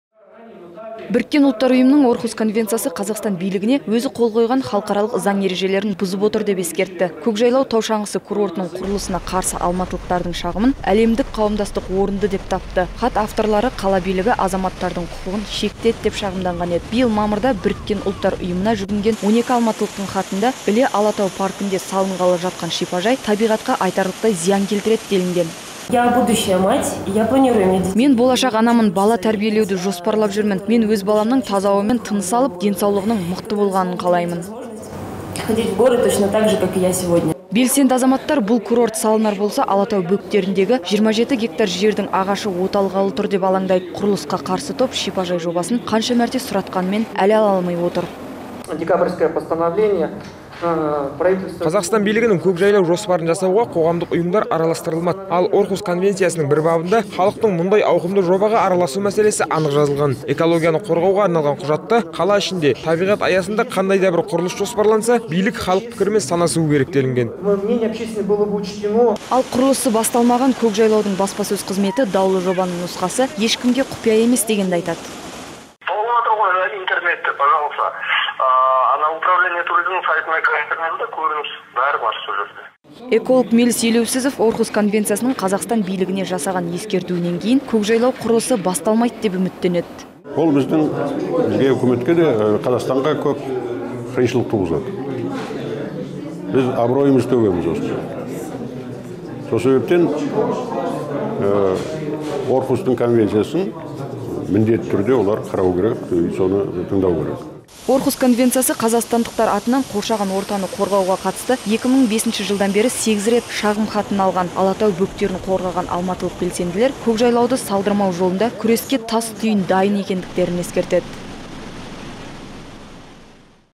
«Көк жайлау» дауы БҰҰ --на жетті
Біріккен Ұлттар Ұйымының Орхусс конвенциясы Қазақстан билігіне өзі қол қойған халықаралық заң ережелерін бұзып отыр деп ескертті. «Көк жайлау» тау шаңғысы курортының құрлысына қарсы алматылықтардың  шағымын әлемдік қауымдастық орынды деп тапты. Хат авторлары «қала билігі азаматтардың құқығын шектеді» деп шағымданған еді. Биыл мамырда Біріккен ұлттар ұйымына жүгінген 12 алматылықтың хатында  Іле алатау паркінде салынғалы жатқан шипажай «табиғатқа айтарлықтай зиян келтіреді» делінген